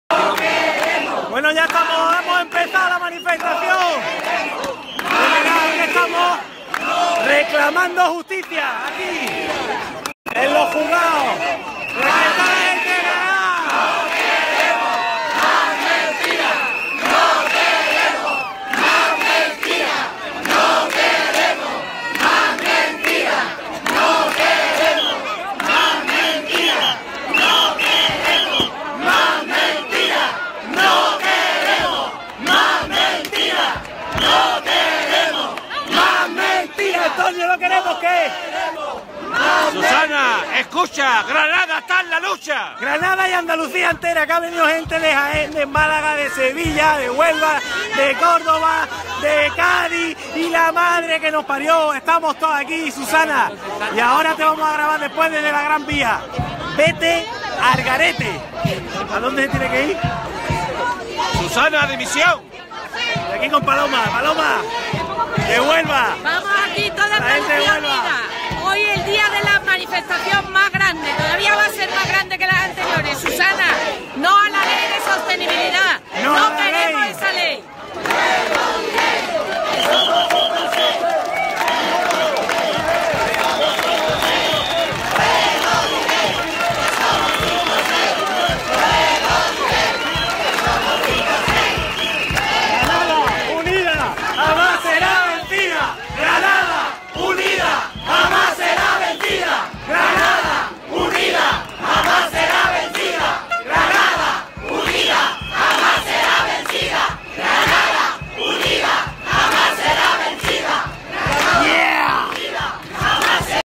Manifestación Granada 15-O
Fuera corruptos de nuestra sanidad. No queremos a las mismas personas de siempre haciendo lo mismo de siempre. No a los recortes y no a las privatizaciones!! Fuera la política de nuestra sanidad pública

Plataforma Teaming de la Asociación Justicia por la Sanidad (para colaborar con 1€ al mes):

—SPIRIBOL—

•Eventos: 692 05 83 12